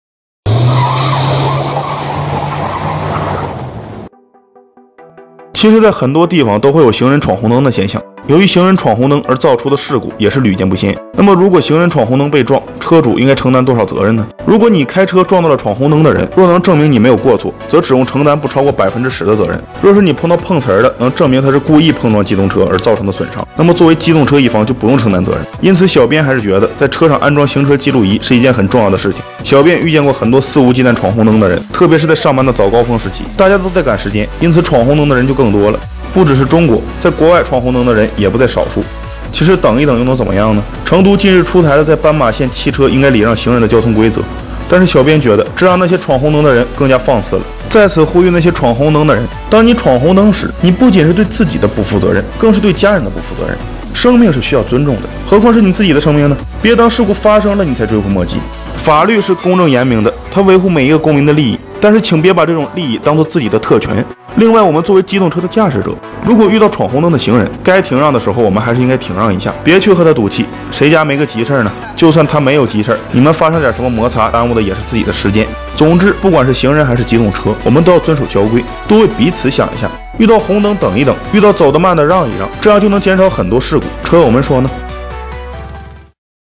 行人闖紅燈被撞，車主應該承擔多少責任？
其實在很多地方都會有行人闖紅燈的現象，由於行人闖紅燈而造成的事故也是屢見不鮮。那麽如果行人闖紅燈被撞，車主應該承擔多少責任呢？